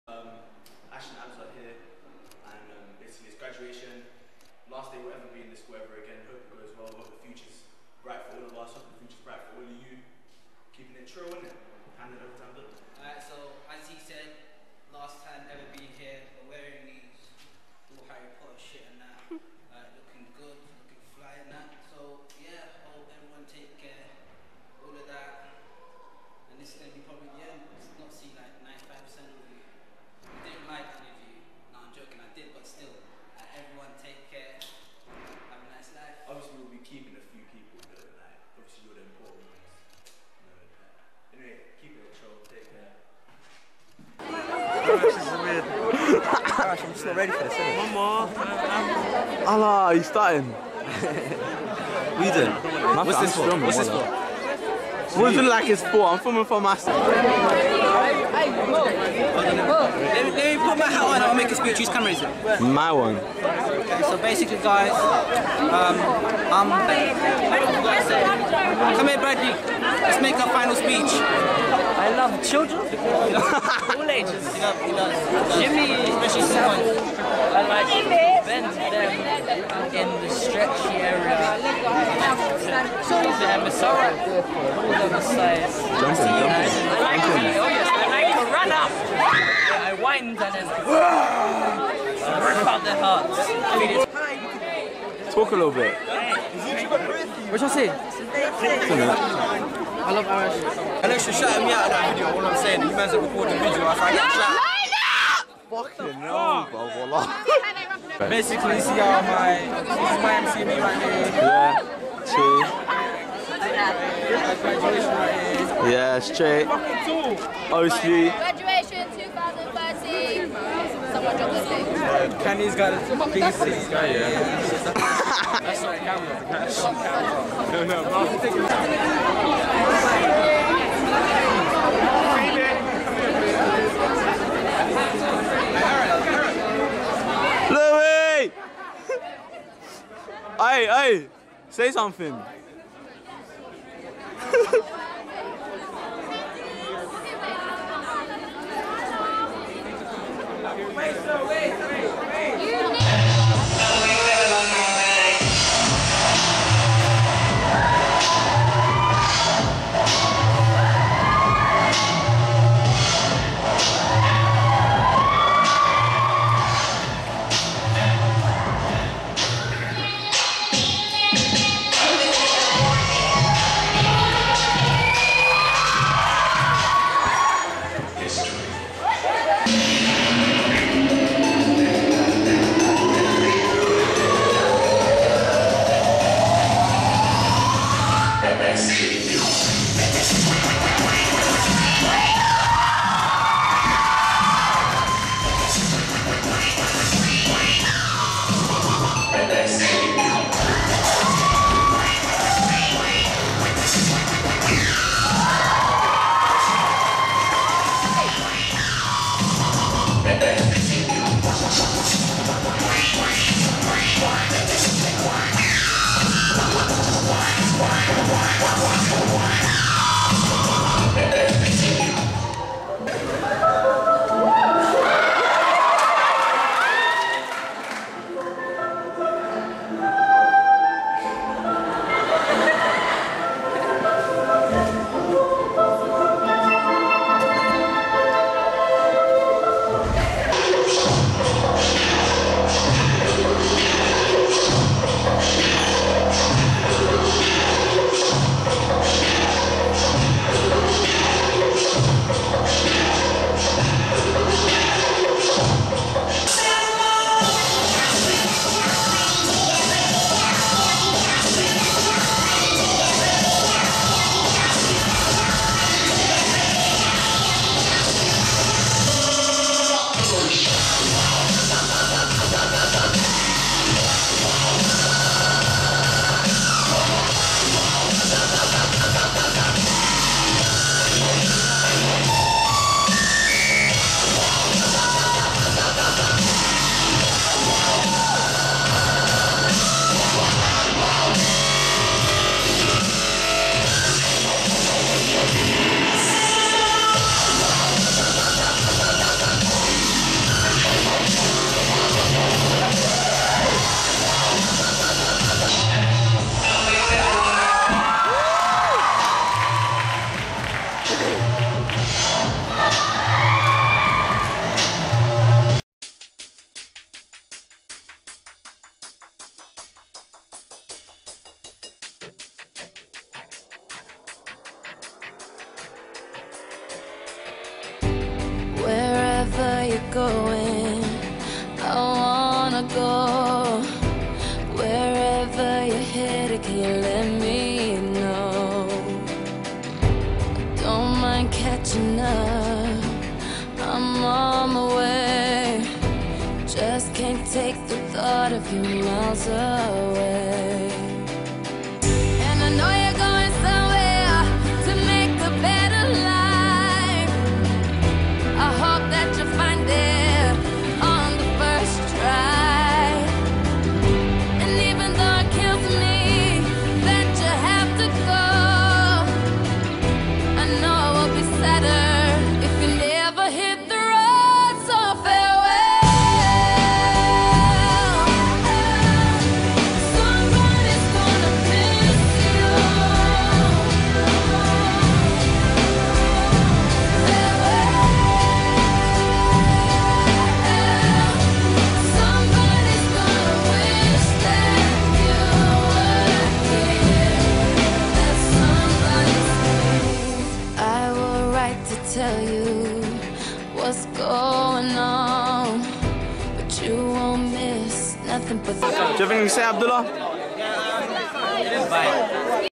Class Of 2013 Graduation Ceremony
After 2 months, Ashh & Abzz come together to start making videos again. Its been a long 5 years at Phoenix but here we are, at the end. Farewell. Twitter: @OfficialAbzz, @ArashTayoub; Facebook: Arash Tayoub